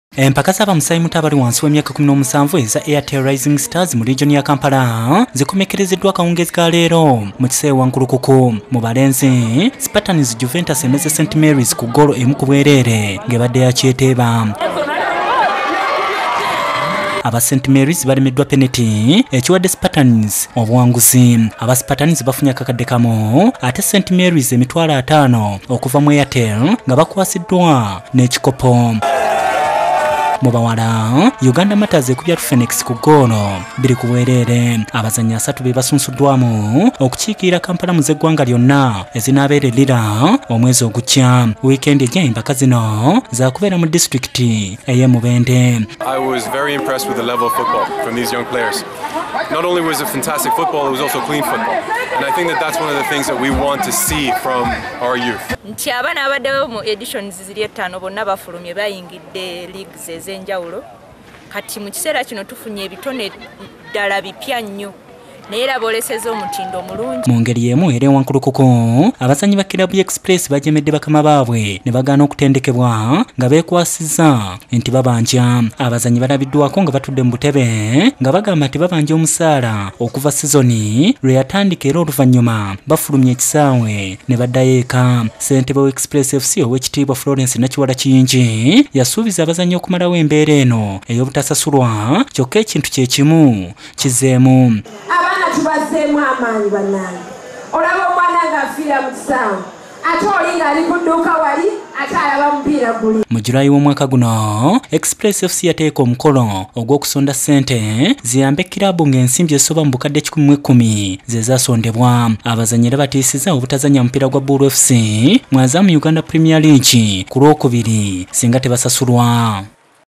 SPORTS:Eza Airtel Rising Stars mu Kampala ziwedde
Spartan ne Uganda Martyrs ziwangudde ebikopo.